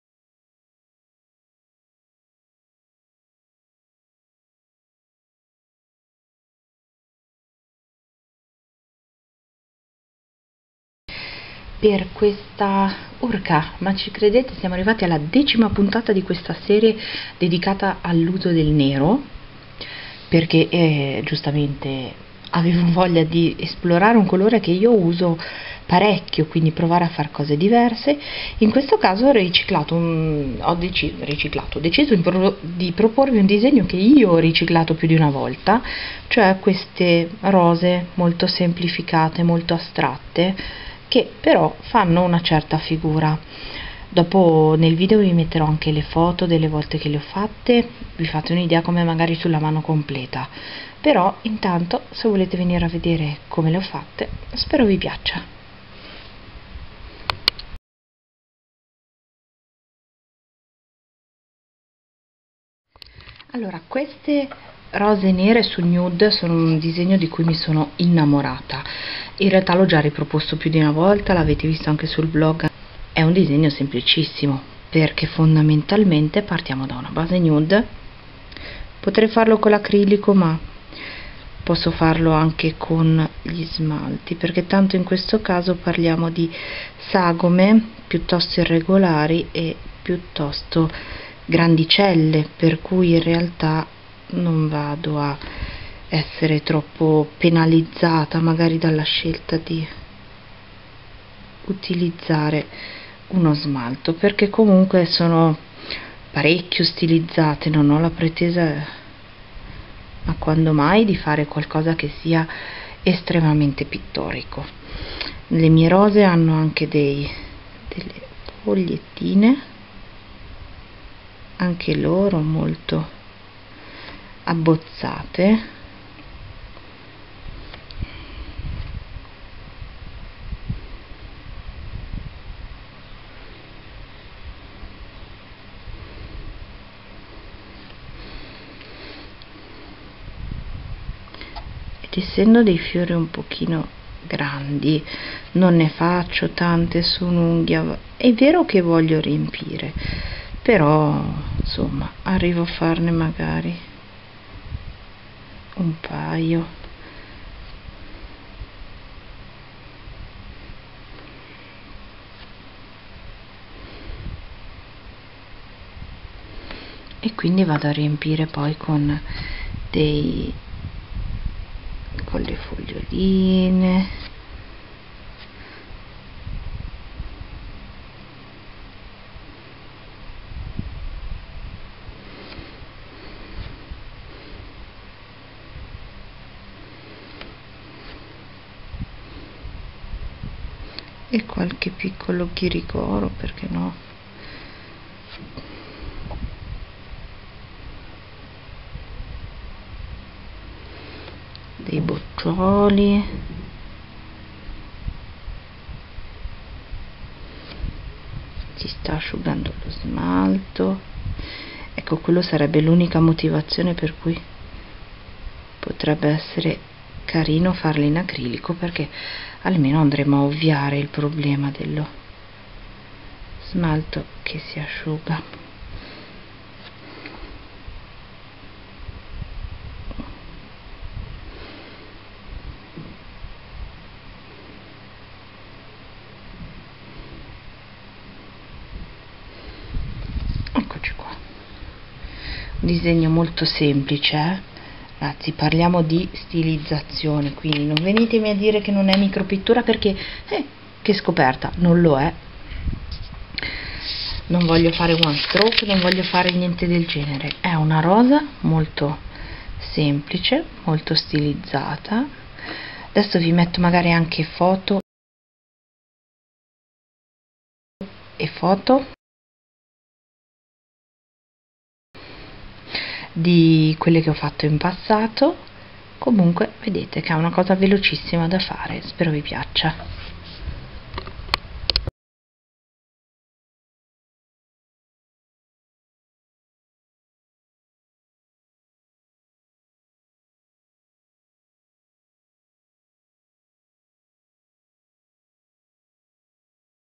The Black Series ep.10 : Black Roses
Una serie di video tutorial con un solo filo conduttore: il colore nero!

Per vedere altre foto e leggere il post di questa nail art... sempre sul blog!

Commenti chiacchere e domande? Qui, oppure su facebook.
o twitter...